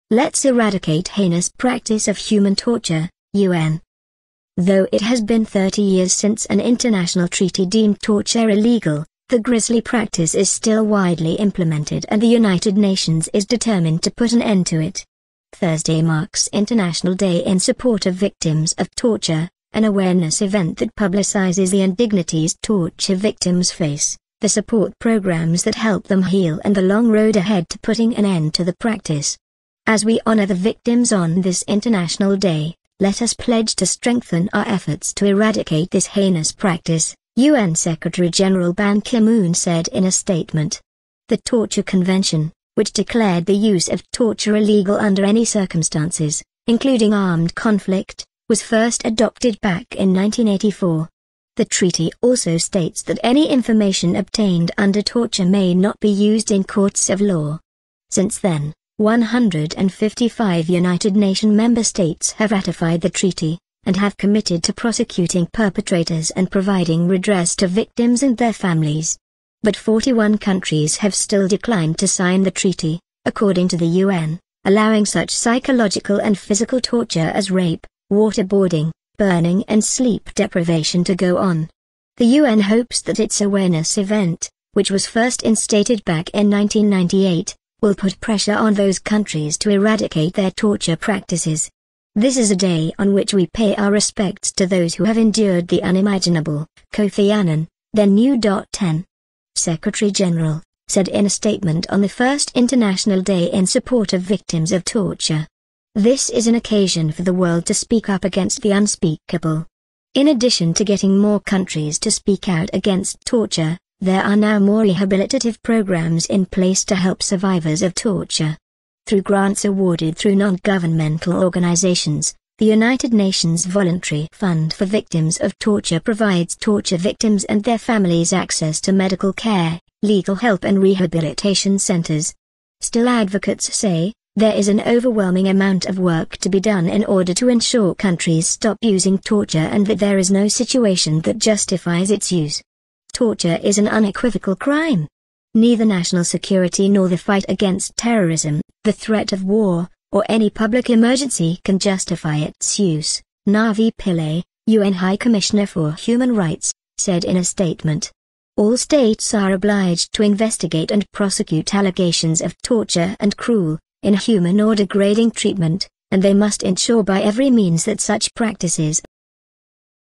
Let's Eradicate 'Heinous' Practice Of Human Torture - UN
Though it has been 30 years since an international treaty deemed torture illegal, the grisly practice is still widely implemented and the United Nations is determined to put an end to it.

Thursday marks International Day in Support of Victims of Torture, an awareness event that publicizes the indignities torture victims face, the support programs that help them heal and the long road ahead to putting an end to the practice.

"As we honor the victims on this International Day, let us pledge to strengthen our efforts to eradicate this heinous practice," U.N. Secretary-General Ban Ki-moon said in a statement.

The Torture Convention, which declared the use of torture illegal under any circumstances -- including armed conflict -- was first adopted back in 1984. The treaty also states that any information obtained under torture may not be used in courts of law.

Since then, 155 United Nation member states have ratified the treaty, and have committed to prosecuting perpetrators and providing redress to victims and their families.

But 41 countries have still declined to sign the treaty, according to the U.N., allowing such psychological and physical torture as rape, water boarding, burning and sleep deprivation to go on.

The U.N. hopes that its awareness event, which was first instated back in 1998, will put pressure on those countries to eradicate their torture practices.

"This is a day on which we pay our respects to those who have endured the unimaginable," Kofi Annan, then-U.N. secretary-general, said in a statement on the first International Day in Support of Victims of Torture. "This is an occasion for the world to speak up against the unspeakable."